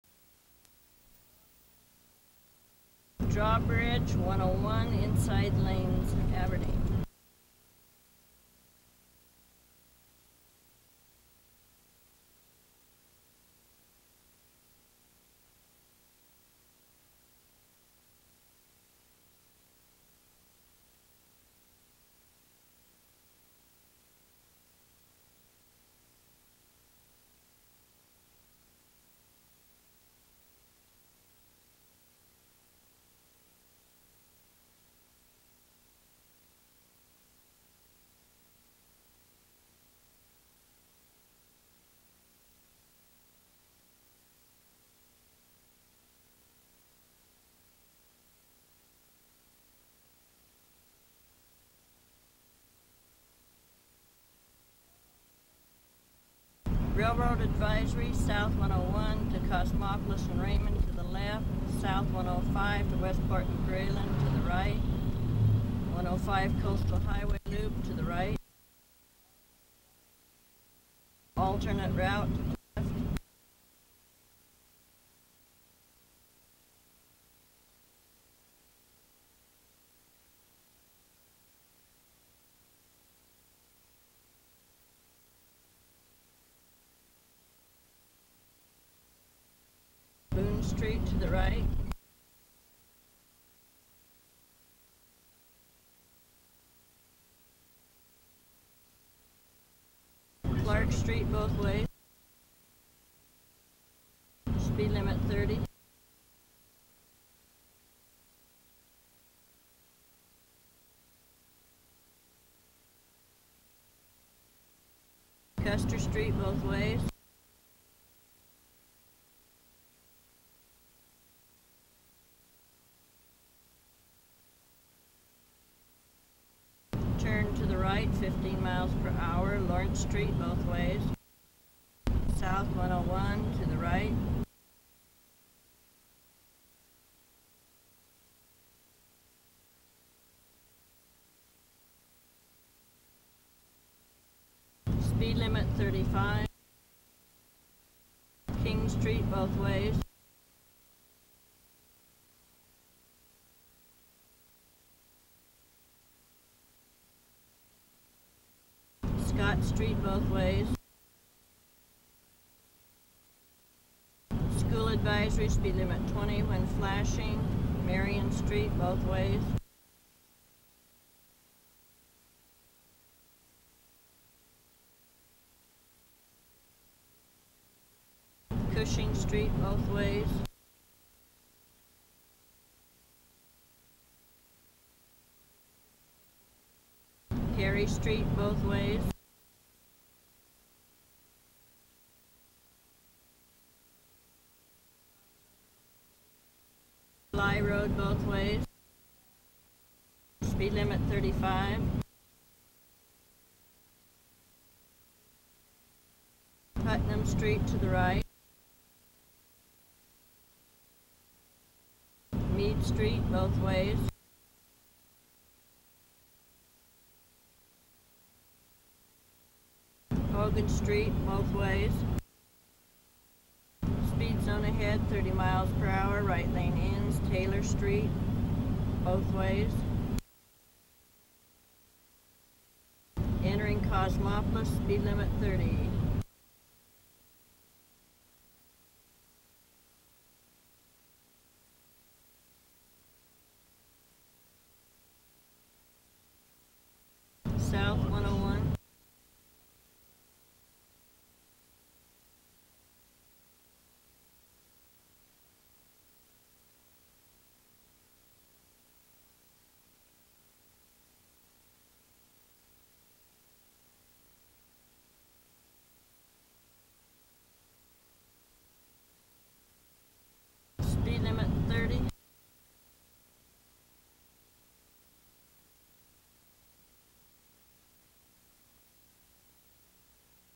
Driving On WA SR-101 South In 1990 (Aberdeen)
Back in 1990, The Washington State Department of Transportation drove South on SR-101 and recorded video to document the highway. This was part of a project that photographed/videotaped the states highways every couple of years. This video now resides at The Washington State Archives who were kind enough to share it.